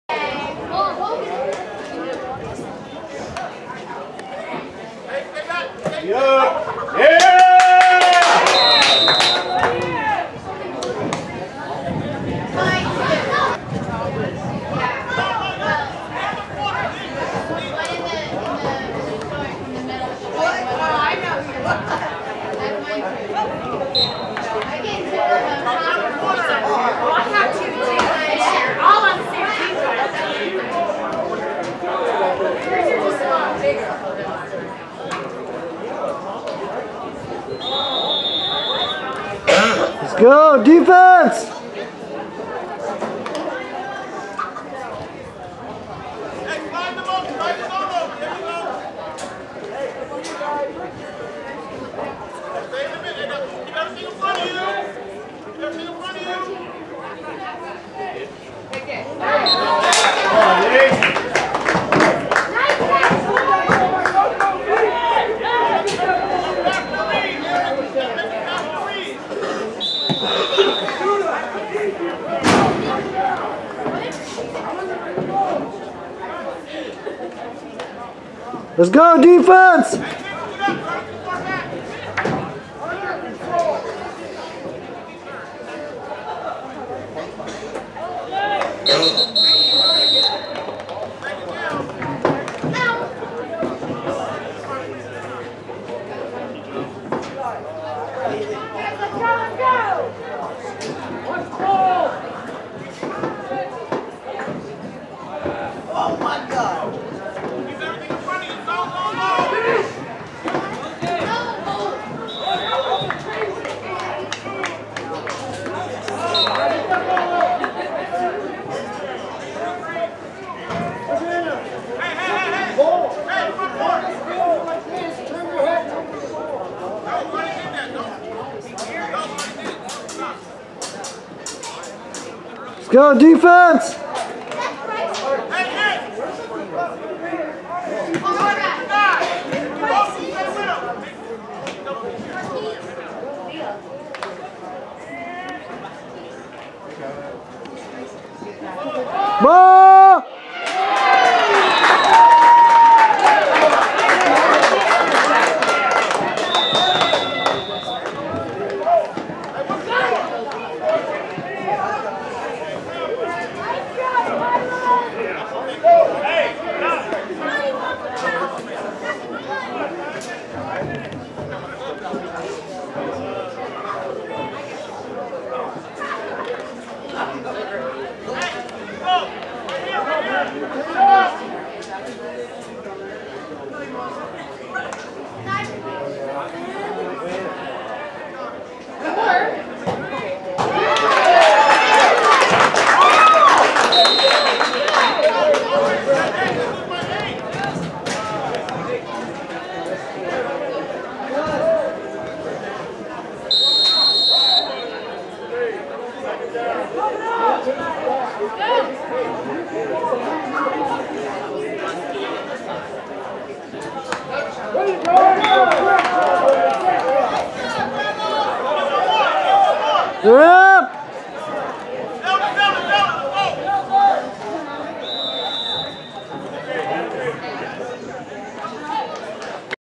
Raptors Bowl 7 on 7 Tournament Auburn Ravens vs Bonney Lake Raptors
Raptors Bowl 7 on 7 Tournament @ Bonney Lake High School on 06/22/2019, hosted by the Bonney Lake Raptors.  This is the 4th game of Pool Play, featuring the Auburn Ravens vs Bonney Lake Raptors.  Aaron was on Offense and Defense for this game.  We won this game , and ended up being the #2 Seed heading into the Playoffs.  This is the final 7 on 7 Tournament for the season.